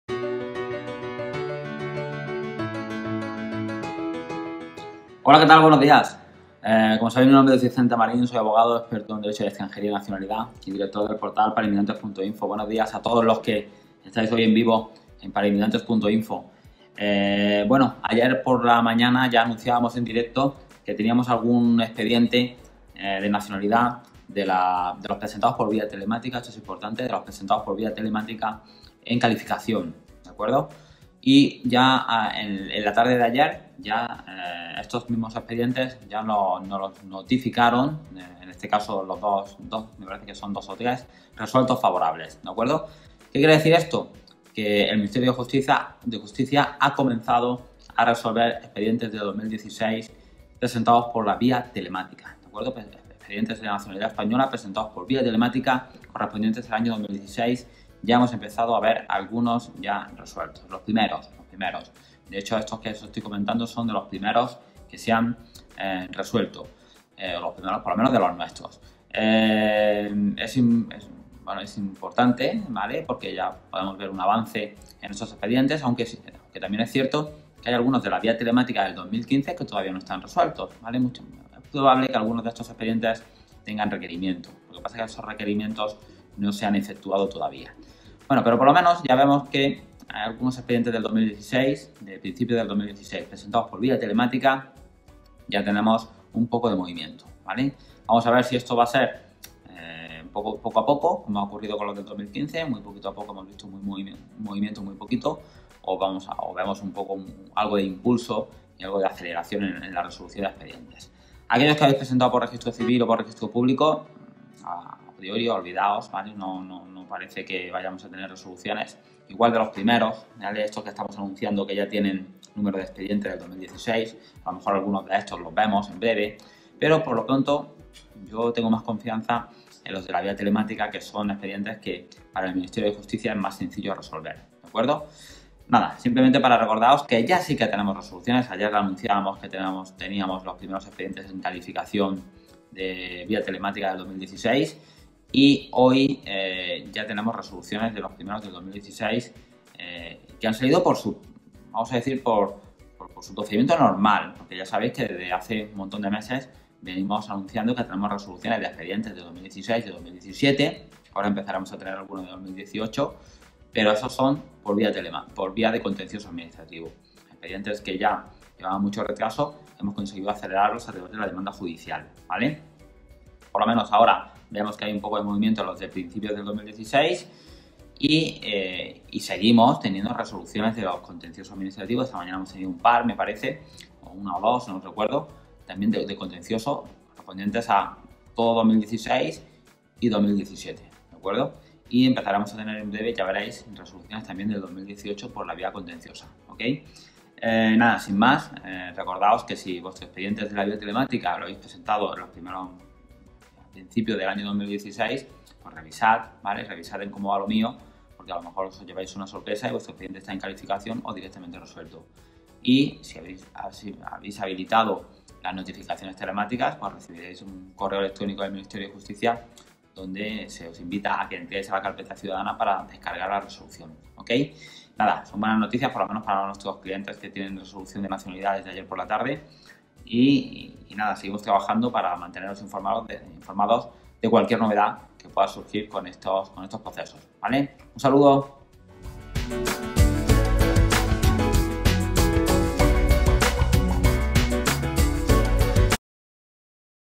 Primeras resoluciones expedientes de nacionalidad "vía telemática" de 2016
Ya tenemos los primeros expedientes de nacionalidad española presentados por vía telemática en 2016 resueltos. Los expedientes presentados por registro civil y registro público durante el año 2016 todavía tendrán que esperar a ser registrados para poder ser resueltos.